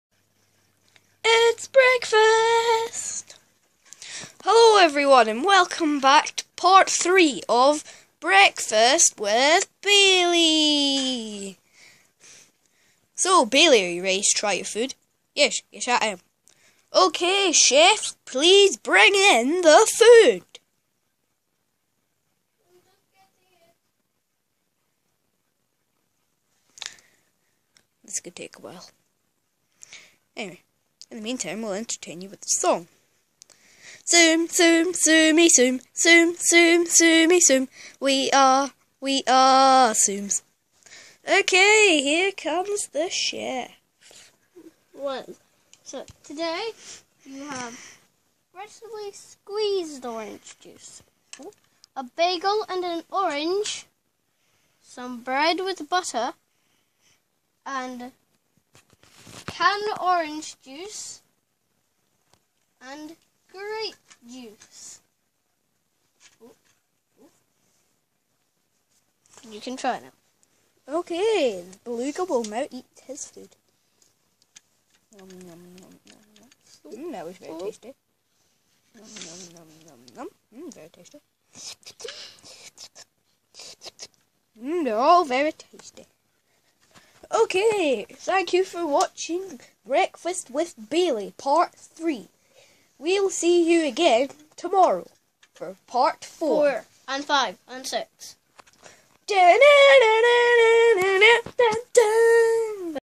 Breakfast with Baillie part 3
Baillie eats a surprise meal chosen by claire this one is very long